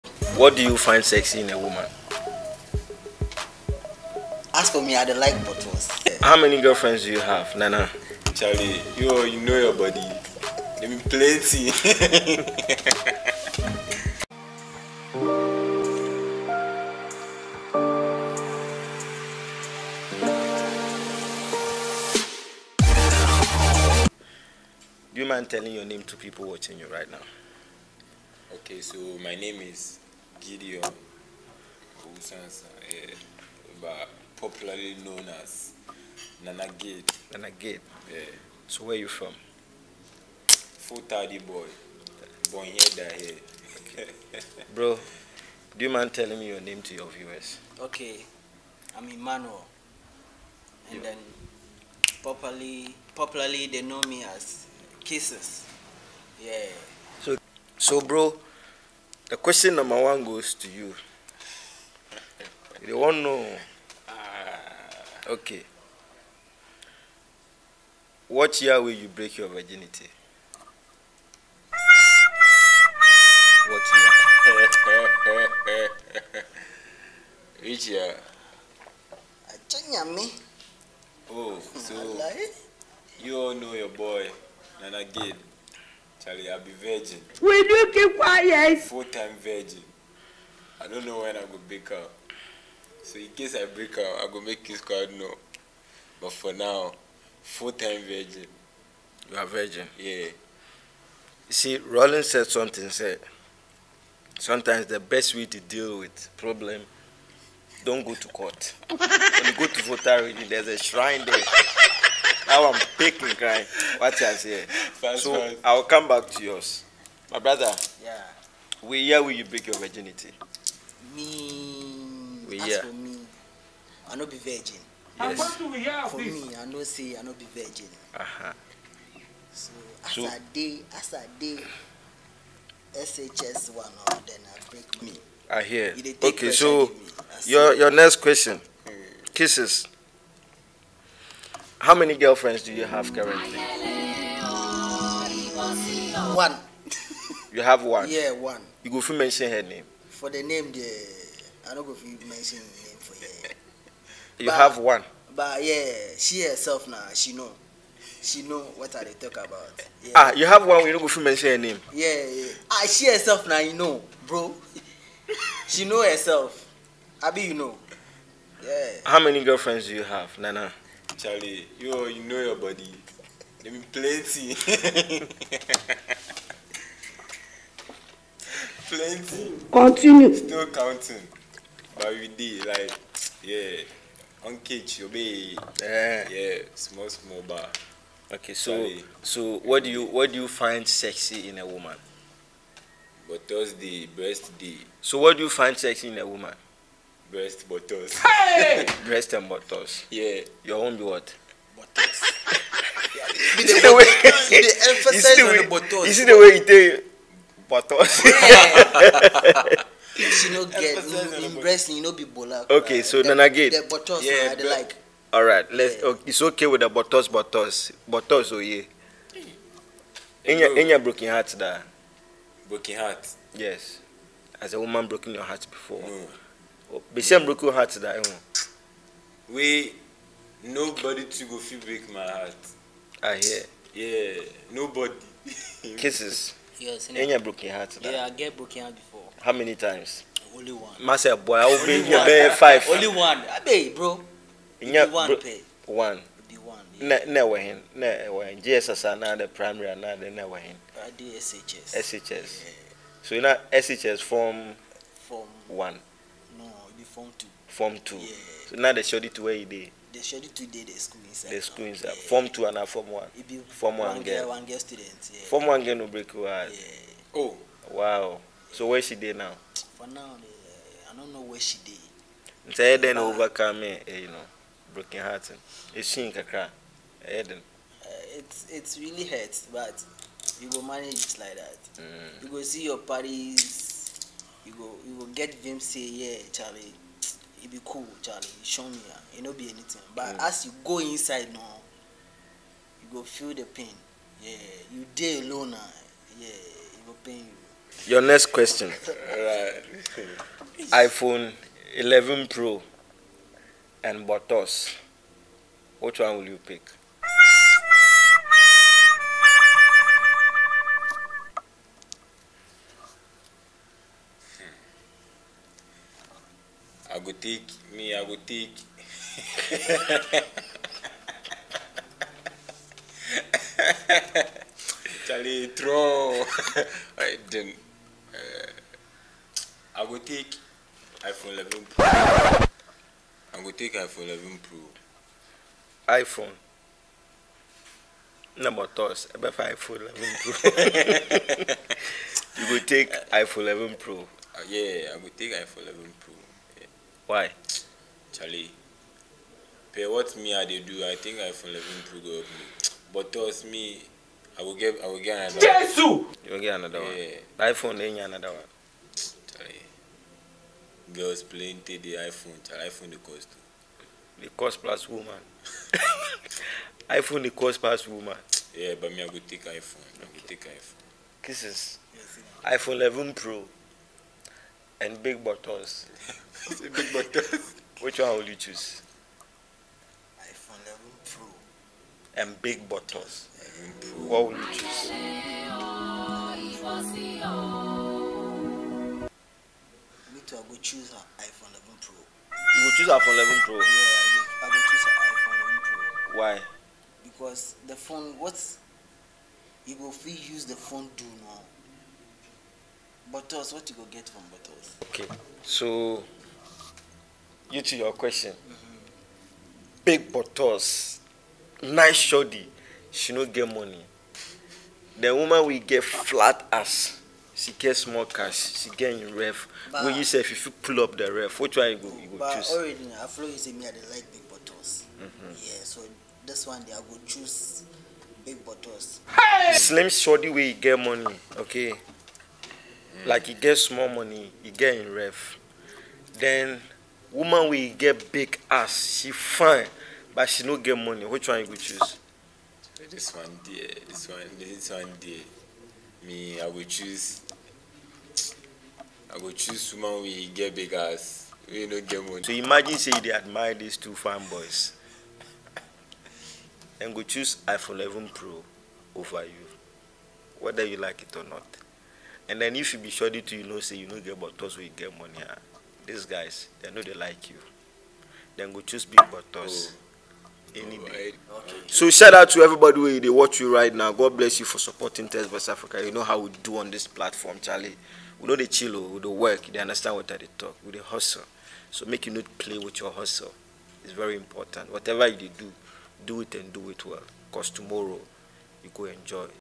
IPHONE 11 PRO MAX vs BIG ASS |what will you choose| BOYS TALK Episode 1
In this video,the pannelist discuss between Iphone 11 Pro Max and Ladies with big ass ,what will be their choice.?
It is interesting the kind of choices and answers they gave.

Iphone 11 Pro Max vs Big ass.